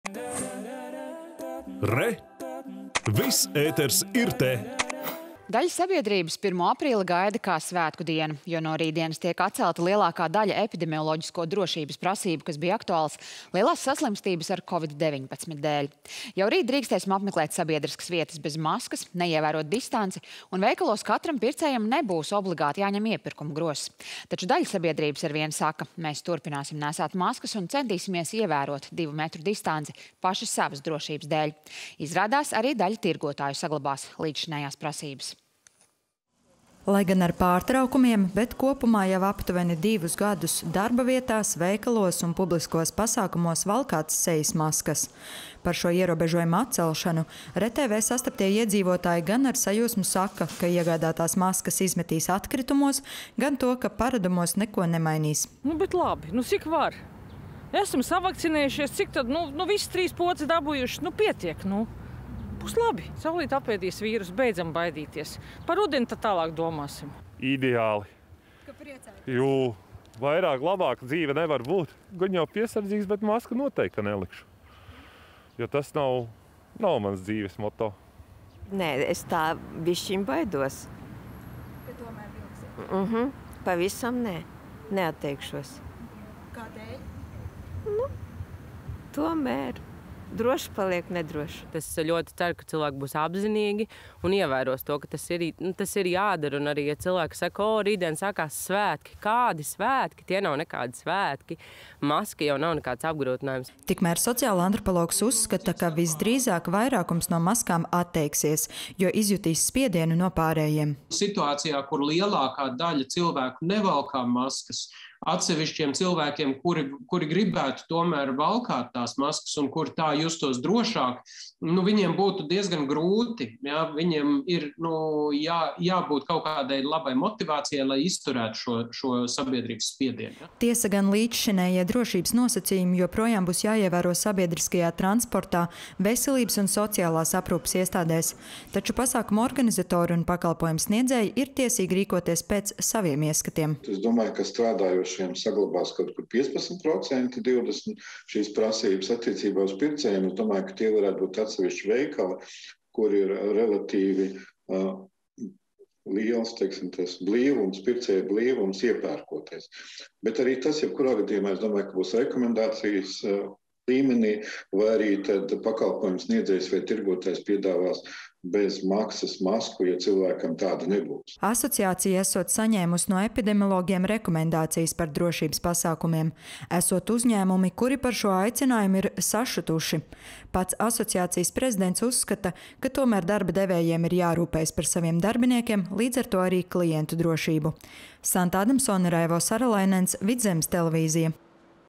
Atceļ kovida drošības pasākumus
Daļa sabiedrības 1. aprīli  gaida kā svētku dienu, jo no rītdienas tiek atcelta lielākā daļa epidemioloģisko drošības prasību, kas bija aktuālas lielās saslimstības ar Covid-19 dēļ. Jau rīt drīkstēsim apmeklēt sabiedriskas vietas bez maskas, neievērot distanci un veikalos katram pircējam nebūs obligāti jāņem iepirkumu grozs. Taču daļa sabiedrības saka – mēs turpināsim nēsāt maskas un centīsimies ievērot divu metru distanci paši savas drošības dēļ. Izrādās, arī daļa tirgotāju saglabās līdzšinējās prasības.